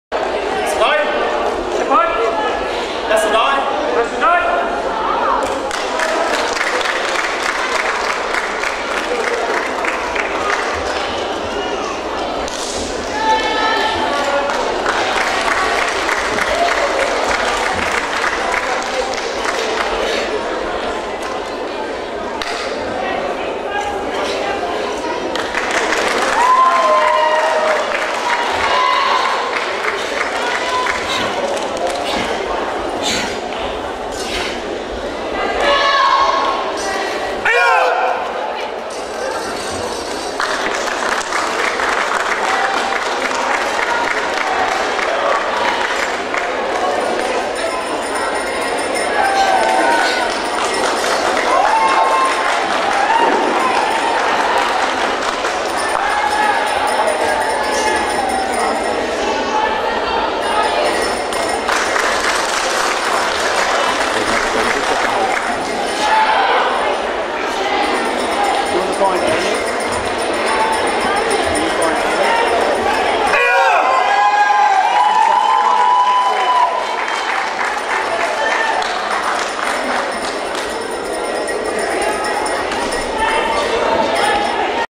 gkr nz nationals 27 10 2007 snr male team kata 02 bassai dai
GKR Karate
New Zealand Nationals
Auckland
Otara Pool and Leisure Centre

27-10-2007

Ringx: Senior Male Team Kata